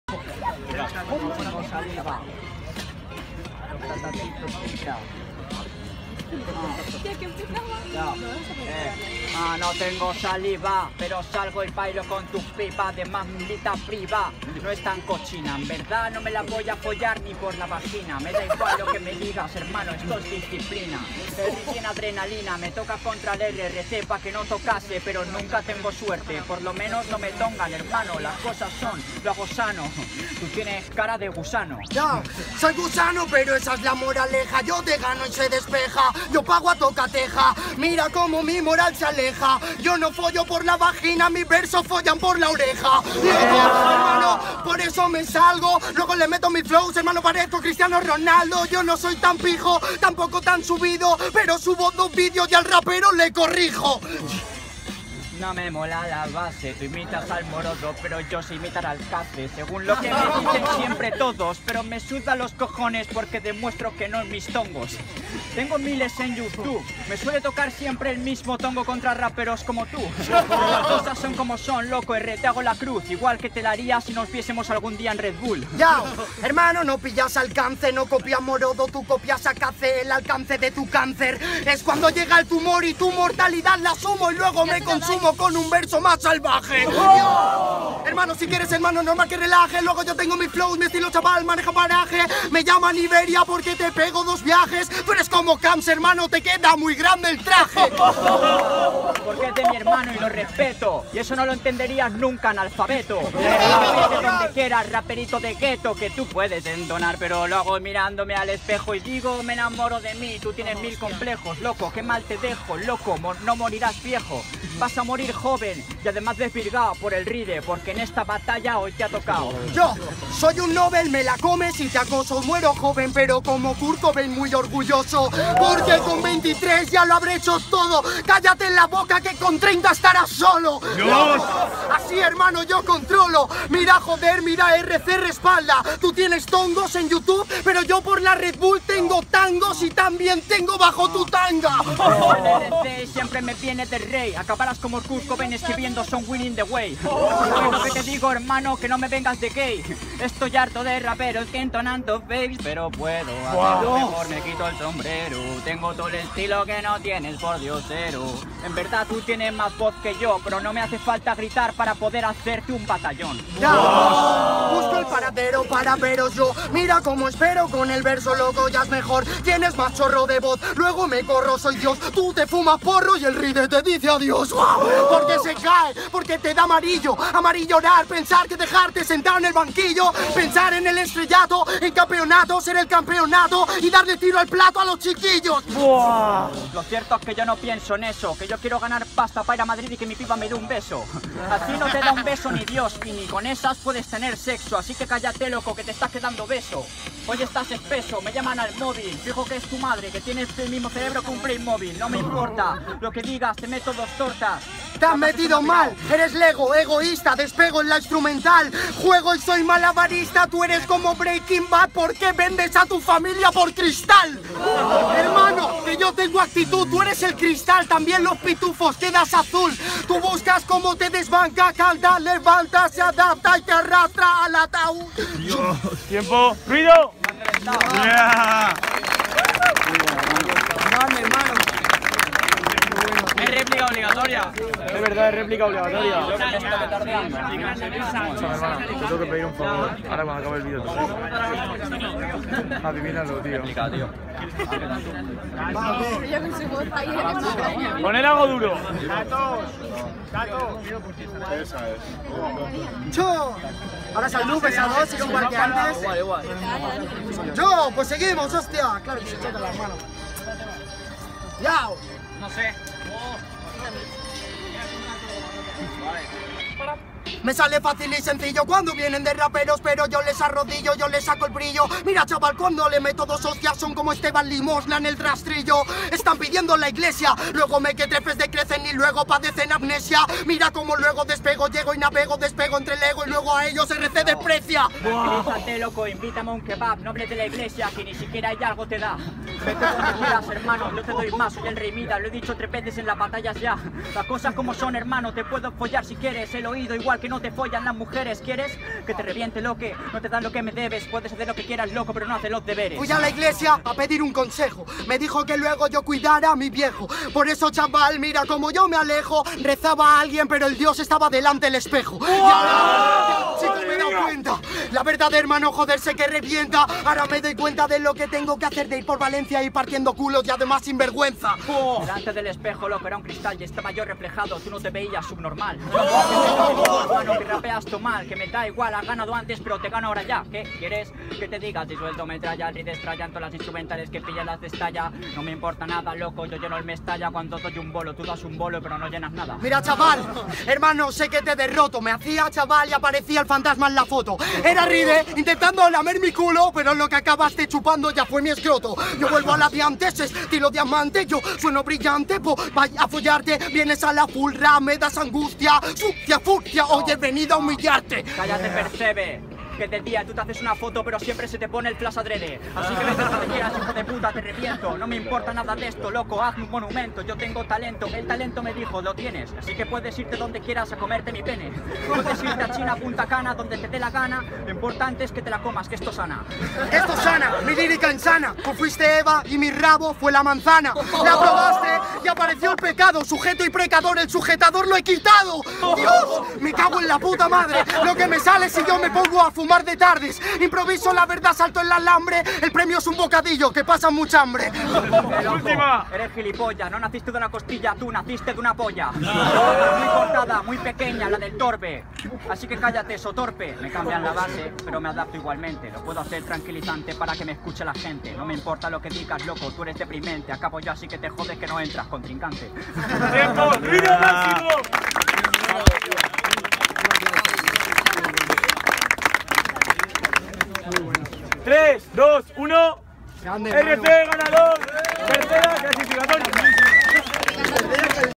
Errece VS Ryde (BATALLÓN) (FINAL) [3a CLASIFICATORIA CV BATTLE]
Ganador de la 3a clasificatoria: ERRECE

Gracias a SETSE clths por los vídeos!!